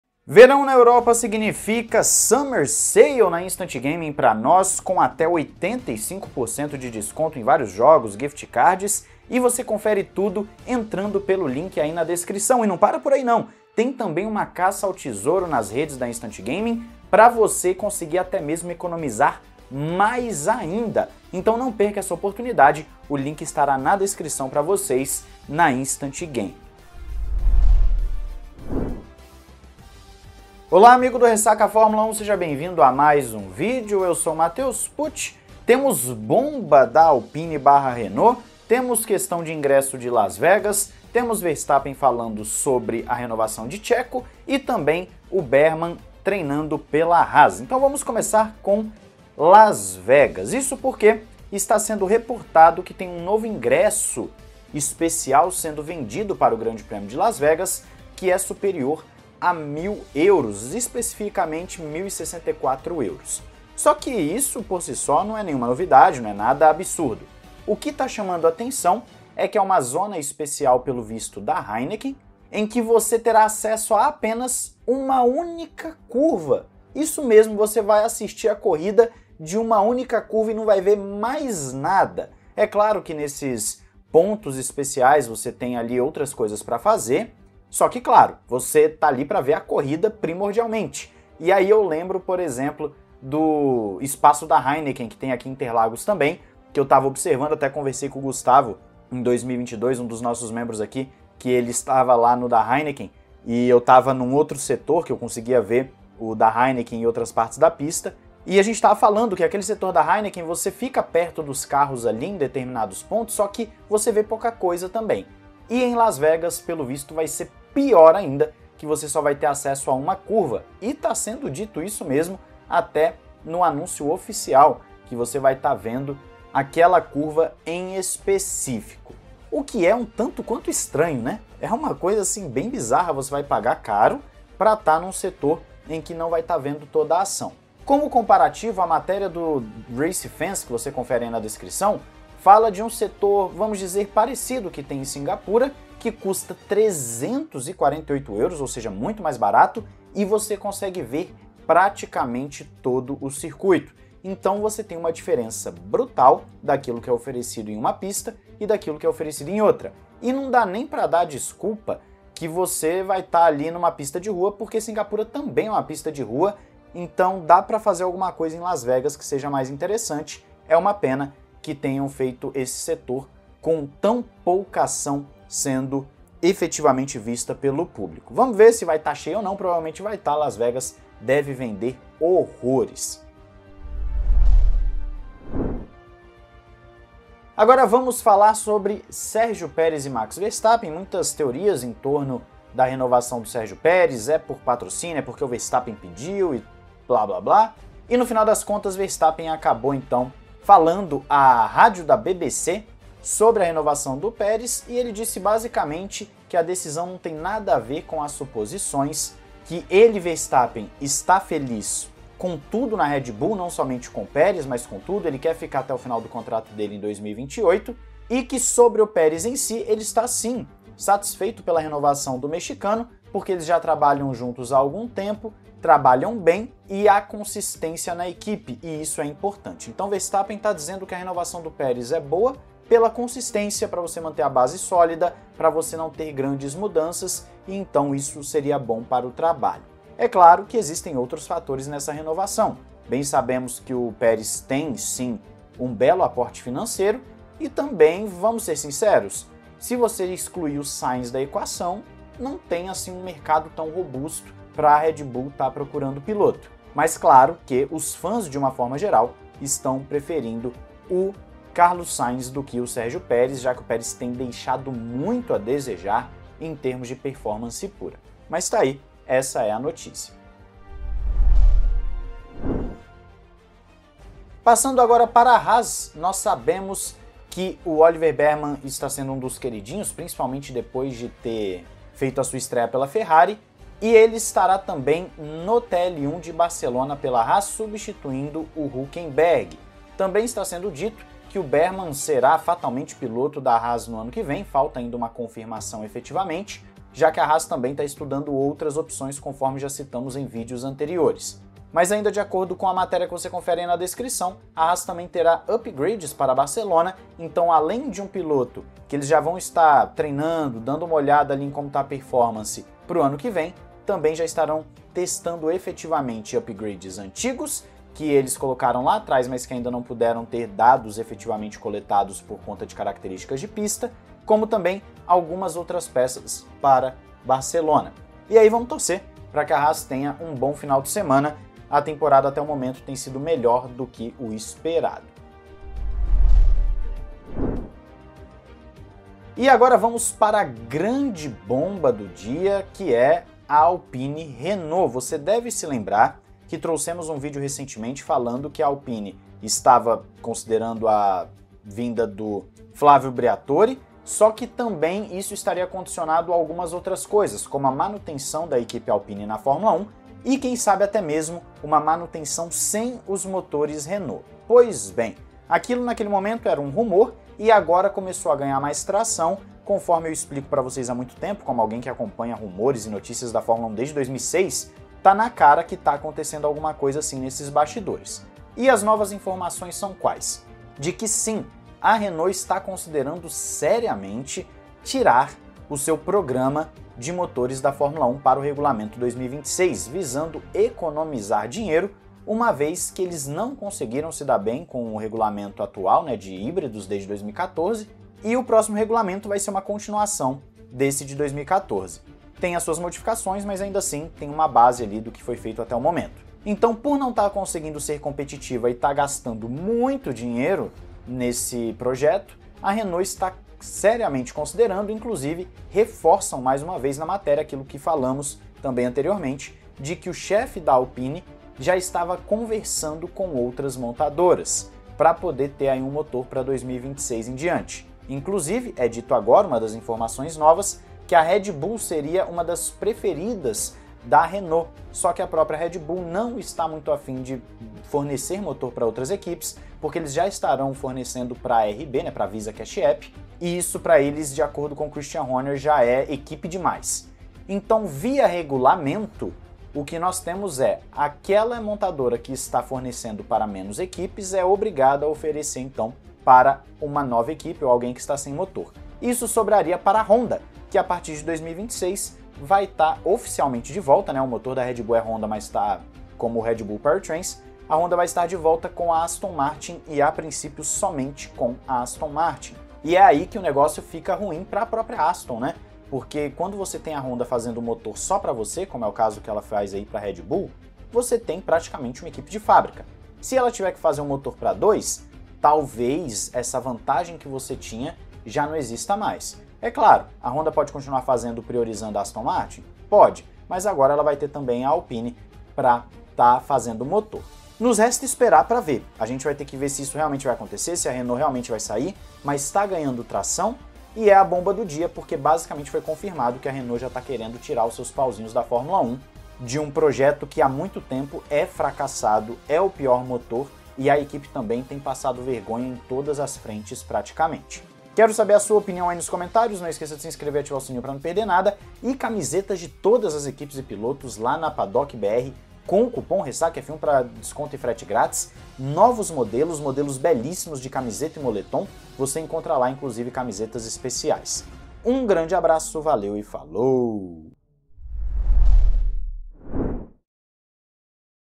B0MBA DO DIA: FIM DO MOTOR RENAULT NA F1 (RUMOR FORTE) / HAAS / RED BULL / LAS VEGAS
Sergio Milani
@scuderiamilani (Twitter)

João 3:16

00:00 - SUMMER SALE até 85% de desconto!
00:50 - Novo ingresso do GP Las Vegas, mas...
03:07 - Max se pronuncia sobre renovação de Checo
04:35 - Bearman testará Haas
05:39 - Alpine vai ficar sem motor...
07:08 - Vai sobrar para Red Bull ou Honda
08:41 - NÃO ESQUEÇA!